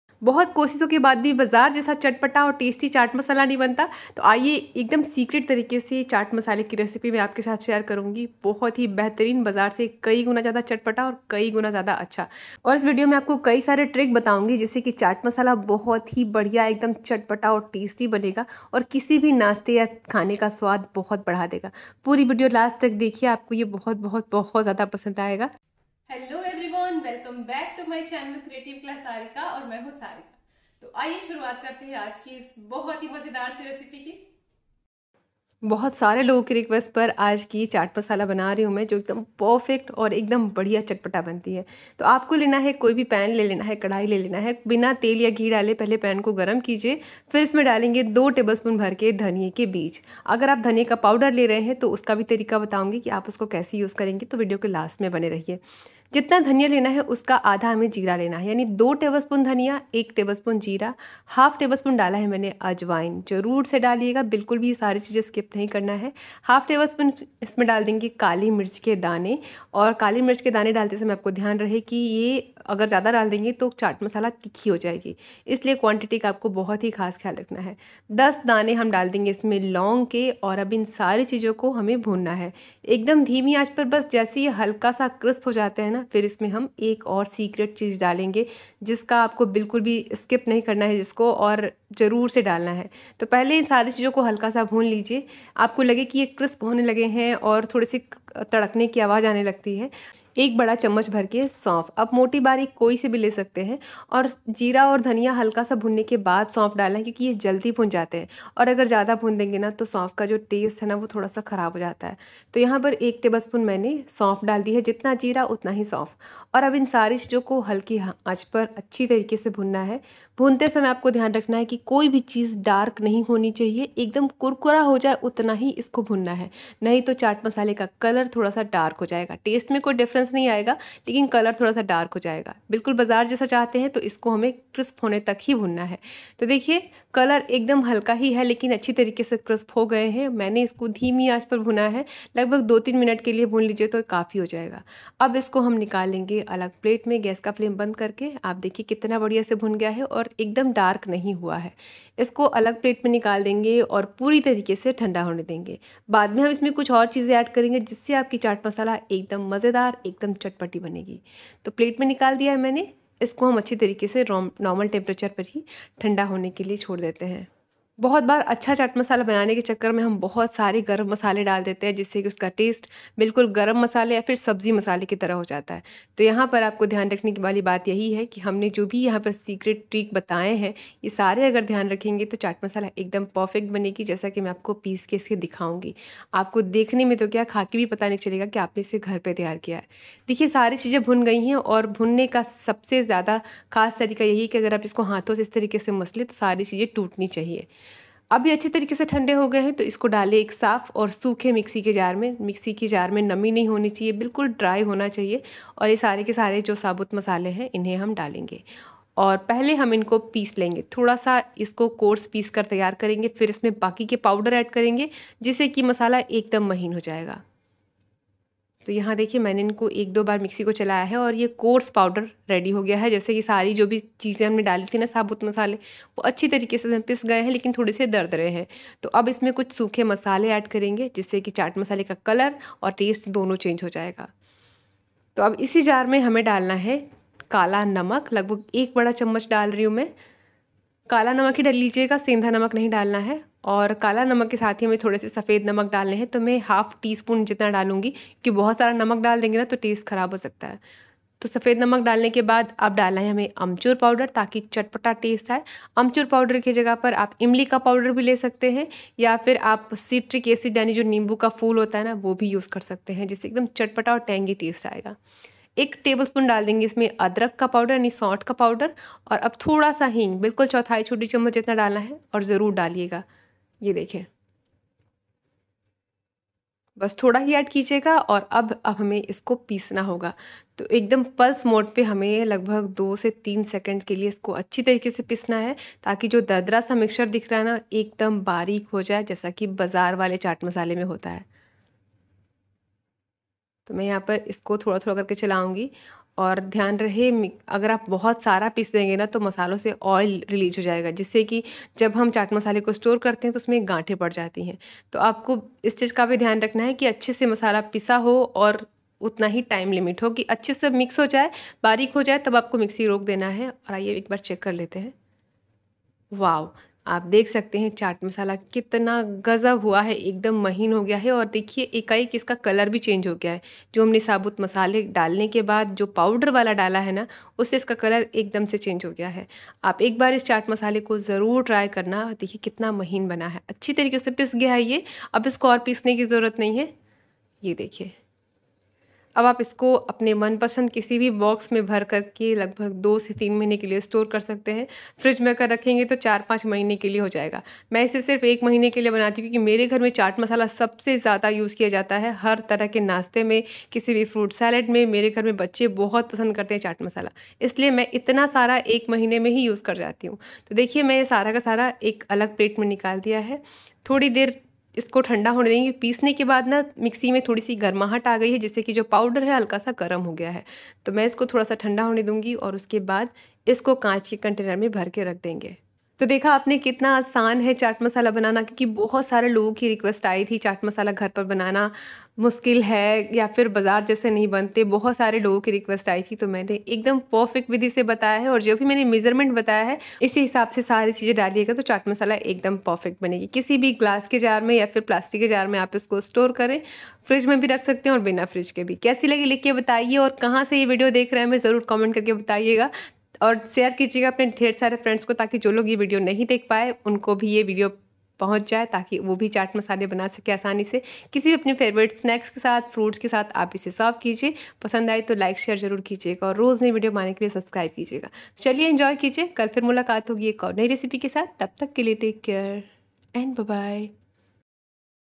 मार्केट जैसा चटपटा चाट मसाला की सेक्रेट विधि देखलिए तो पूरे साल केलिए बनालेगे chat masala recipe
Published On 13June,2024